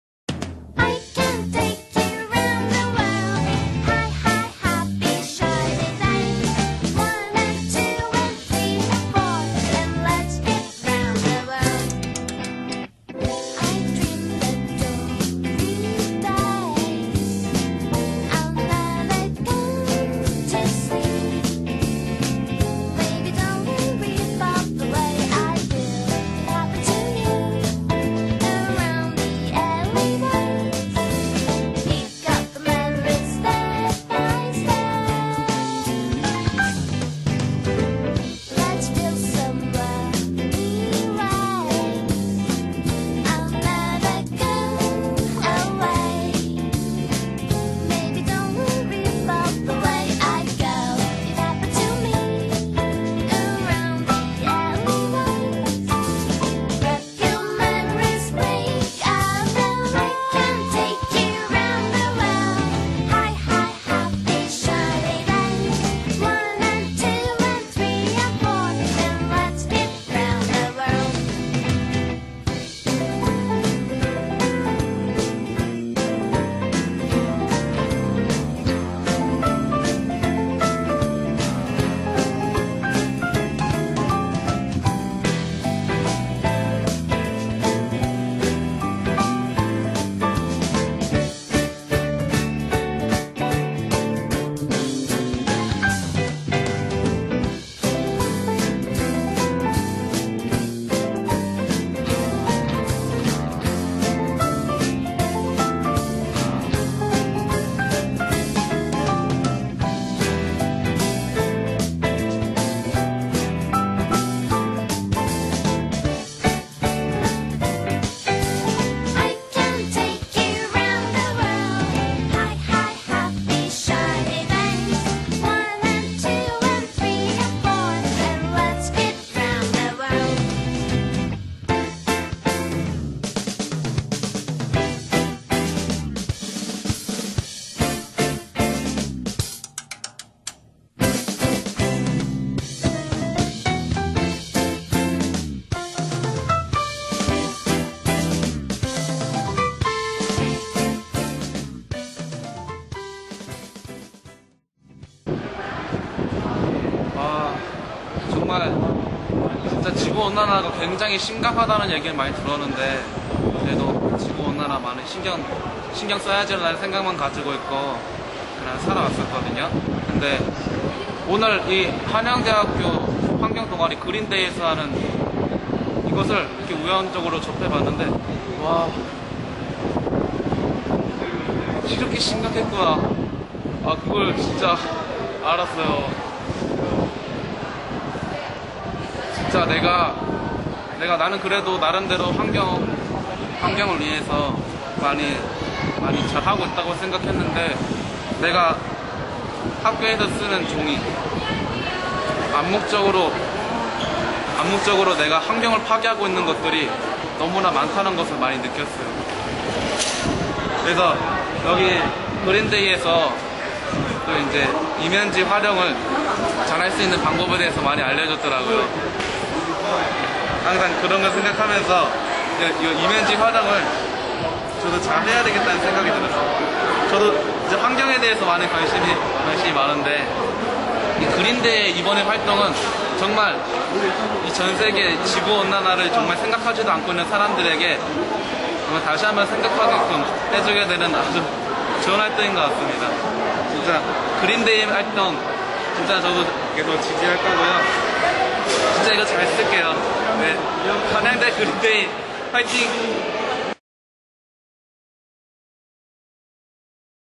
한양대학교 환경동아리 Green day의 Green note 캠페인
대자연 한국그린캠퍼스대학생연합회 소속 한양대학교 환경동아리 Green Day!
Green note노트 캠페인을 통해 그린지구를 만들어 가고 있습니다.